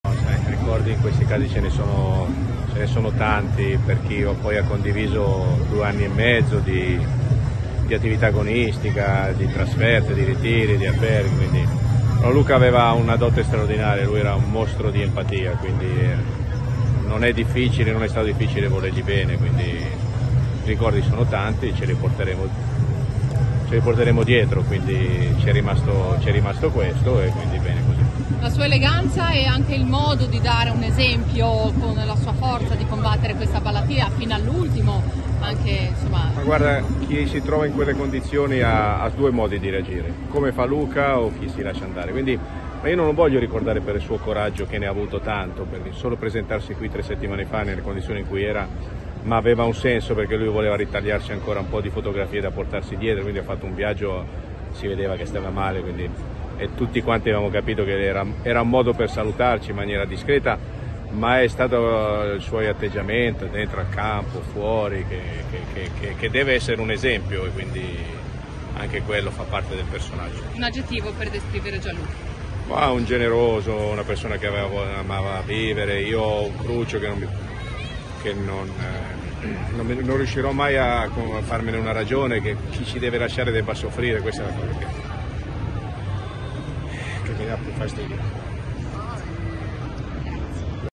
Addio Vialli, il ricordo commosso di Dossena
(Agenzia Vista) - Genova, 6 Gennaio 2023 - "Ci sono tanti ricordi abbiamo condiviso due anni e mezzo di attività agonistica, era un mostro di empatia e non era difficile volergli bene". Le parole di Giuseppe Dossena, ex compagno di squadra nella Sampdoria di Gianluca Vialli che vinse lo scudetto del 1991. / Ufficio Stampa Regione Liguria